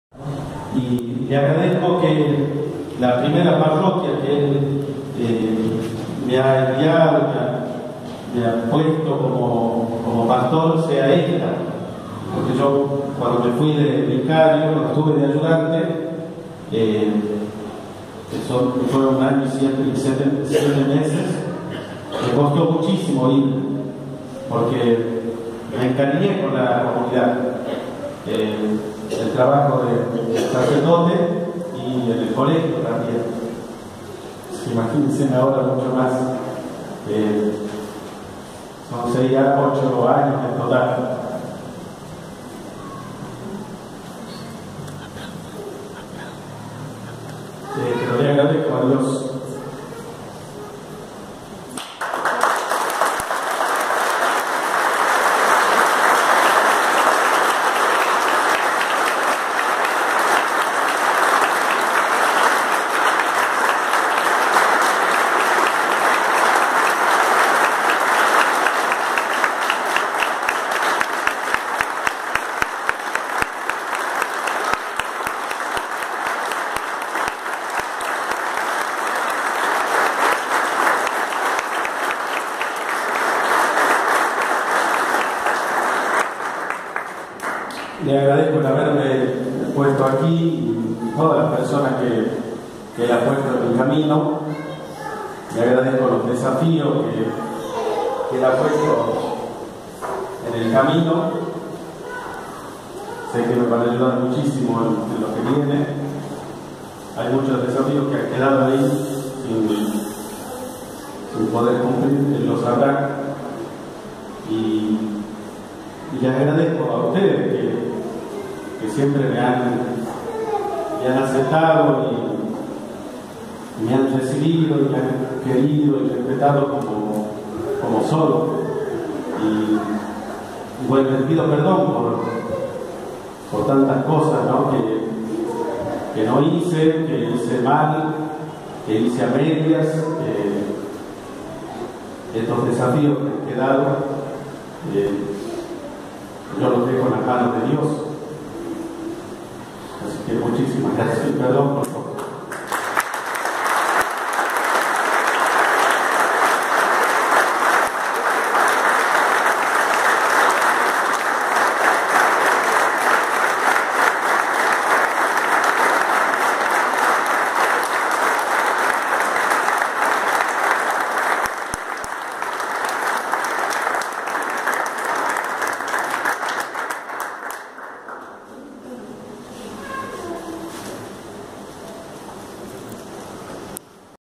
Parroquia San Roque: despedida del Padre Gustavo Flores...
Palabras del Párroco Gustavo Flores, se despide de Recreo. (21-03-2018).